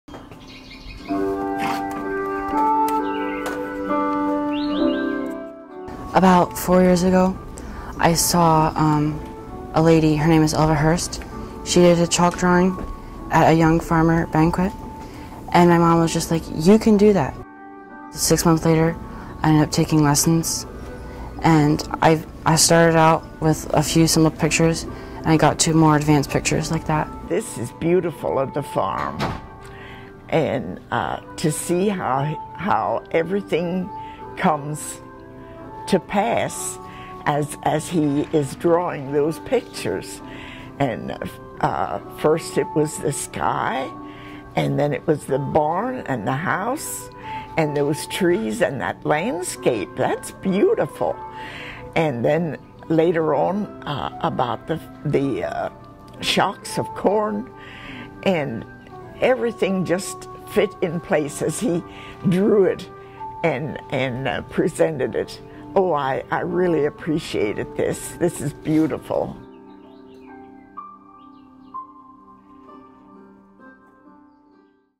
Giving the gift of art: Thomas Herr
Thomas Herr, a young dairy farmer from Central PA, is also an artist. When he's not at home on Philhaven Farm, he loves to share his gift of chalk drawing with local nursing home residents.